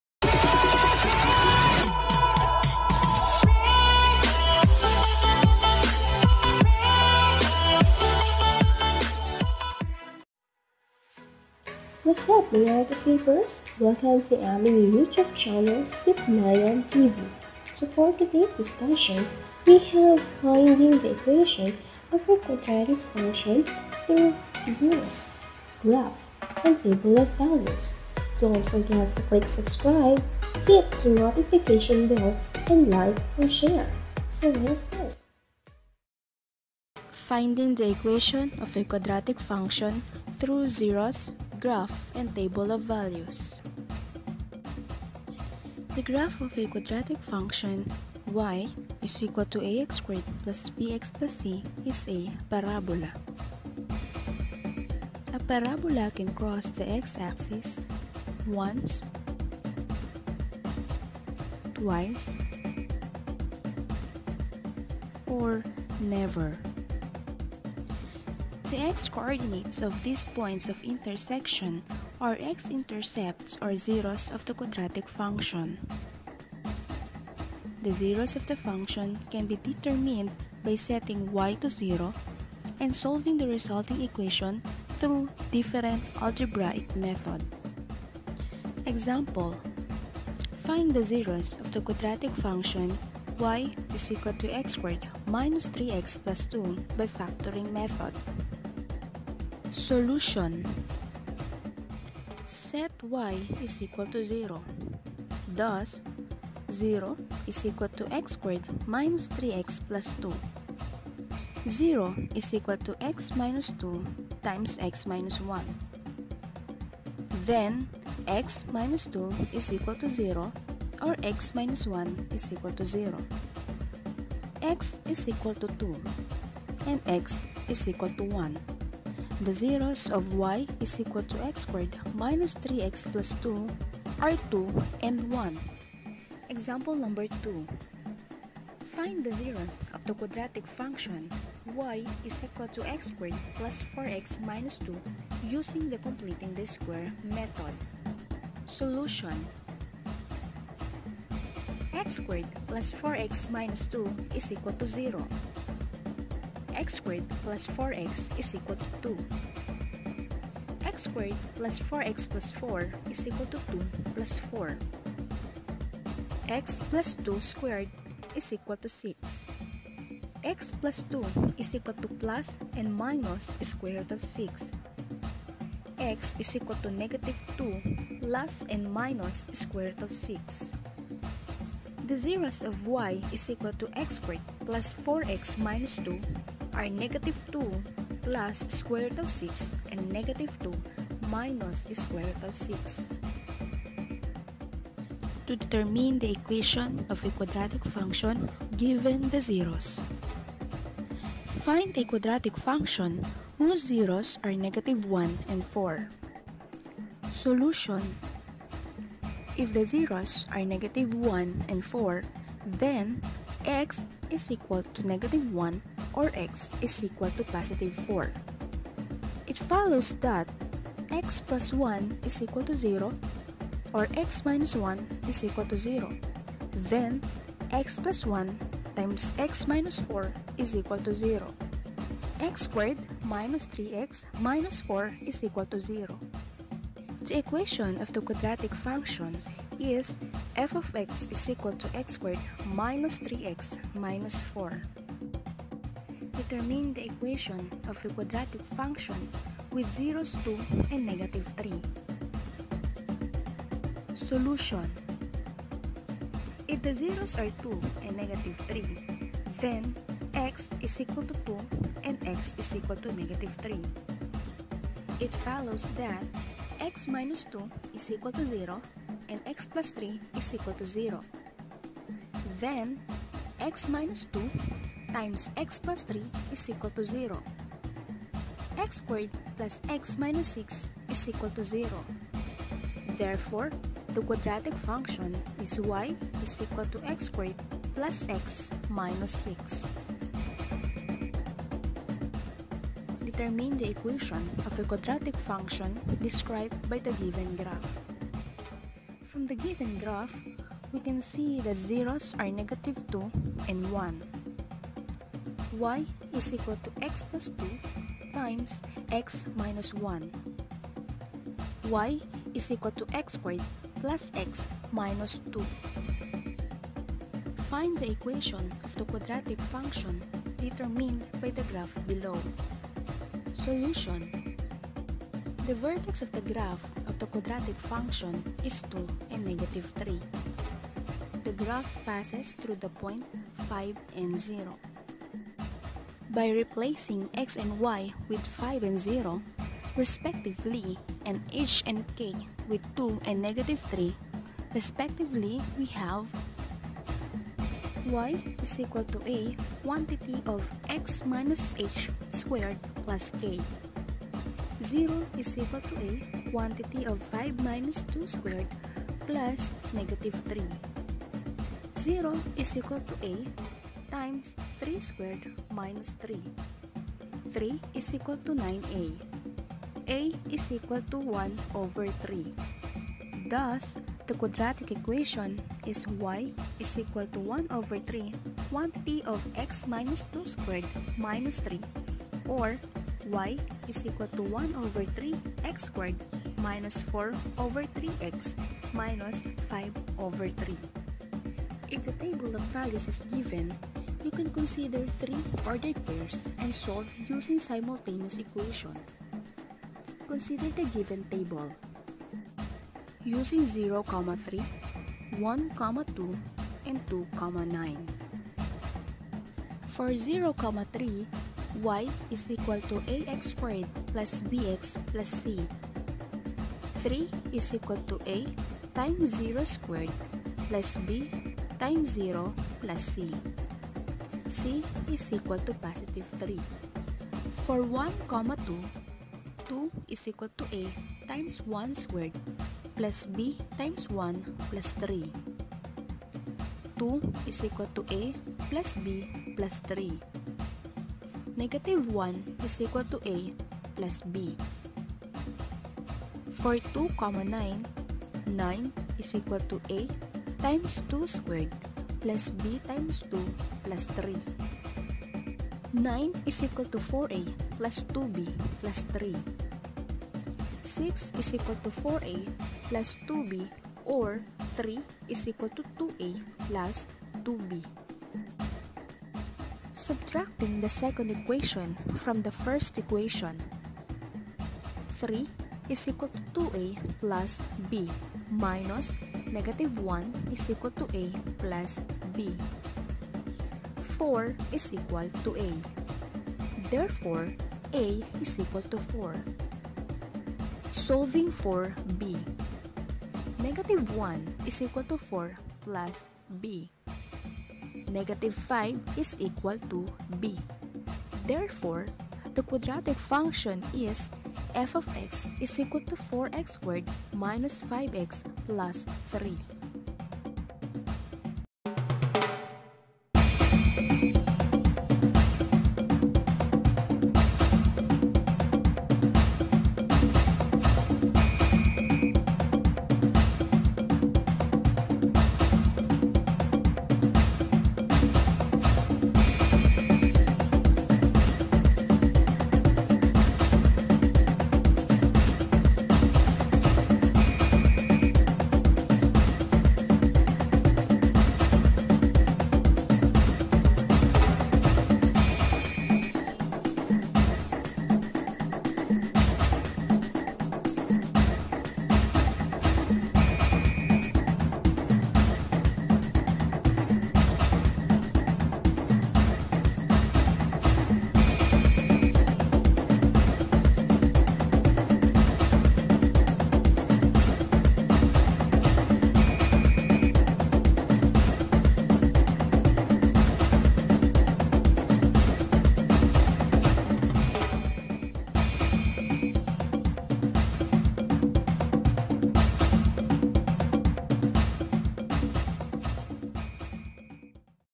Finding the Equation of a Quadratic Function through a. Zeros b. Graph c. Table of Values
Hello mga kaSIPerss! Watch our new uploaded video. It will help you to find the equation of a quadratic function through zeros, graph, and table of values.